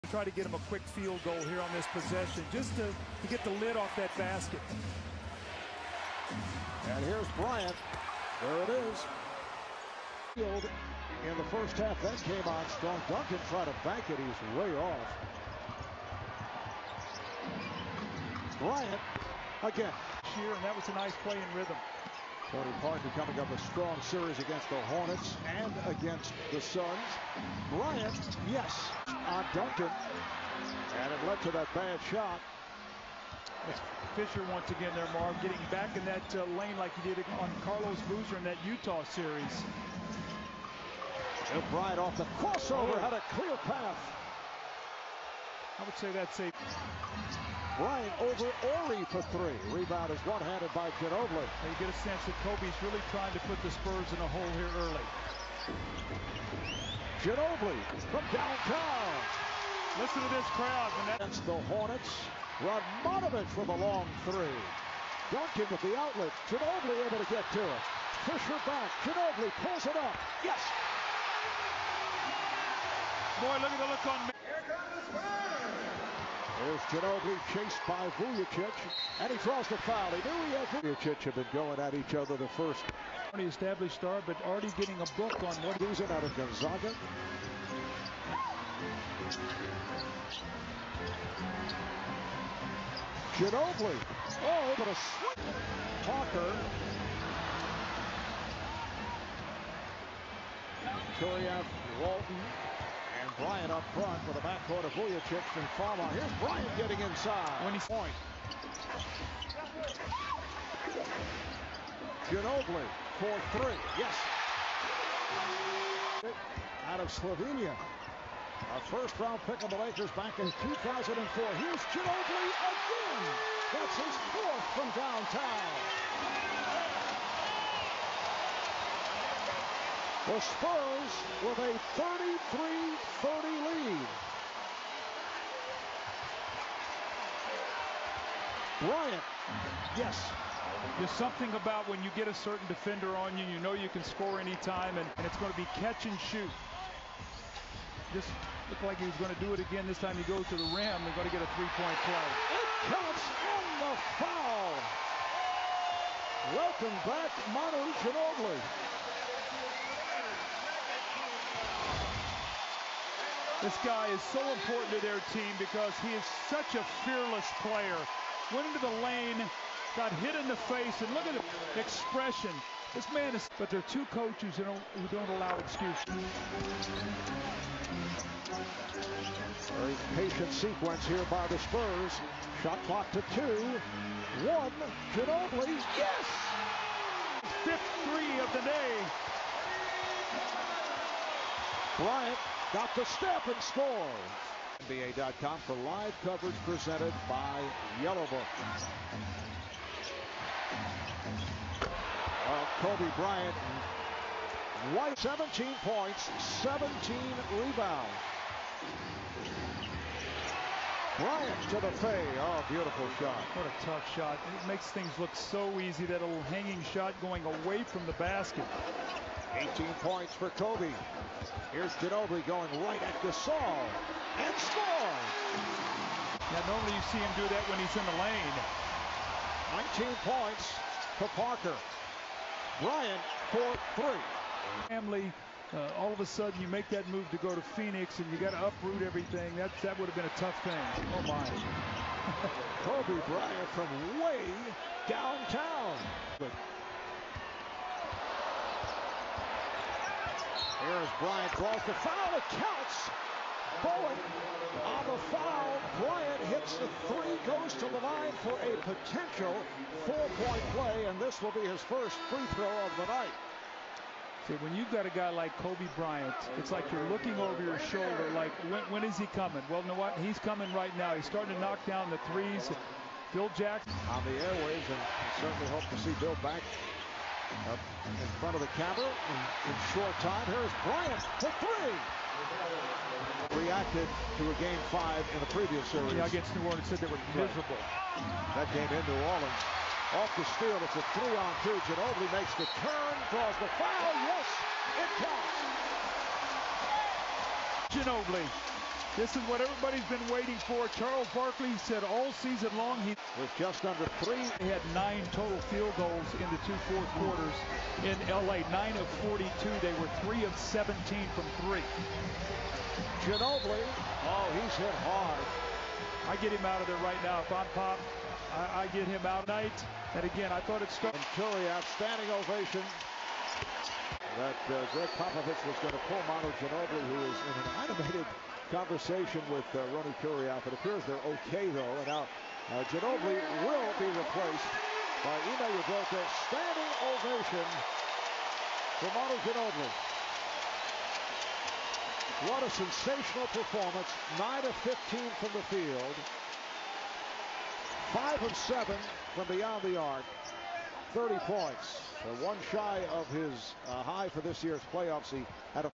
NBA’s Greatest Battles: Kobe Bryant (30pts) vs Manu Ginóbili (30pts) (2008 WCF Game 3)
Kobe Bryant and the Lakers visit San Antonio up 2-0 in the 2008 WCF looking to take a commanding 3-0 lead. However Manu, Duncan, Parker, and role players like Brent Barry and Fabricio Oberto came ready and blew them out of the water. Kobe has 30, but no one else had a good game for the Lakers.